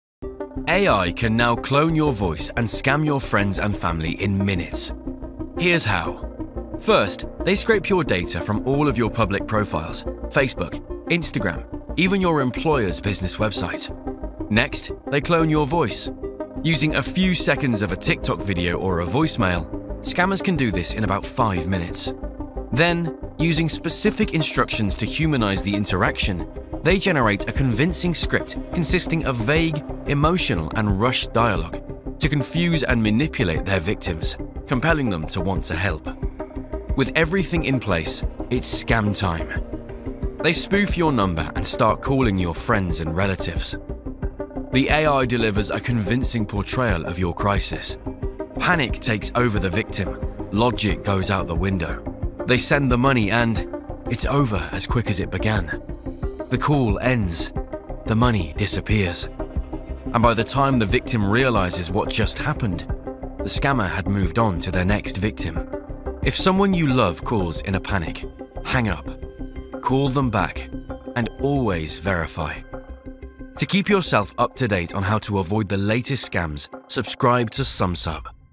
AI Can Clone Your Voice and Scam Your Family in Minutes
Scammers are now using AI to clone your voice and trick your loved ones.
Here’s how it works — and what you need to do to protect yourself.

Sumsub — empowering compliance and anti-fraud teams to fight money laundering, terrorist financing, and online fraud.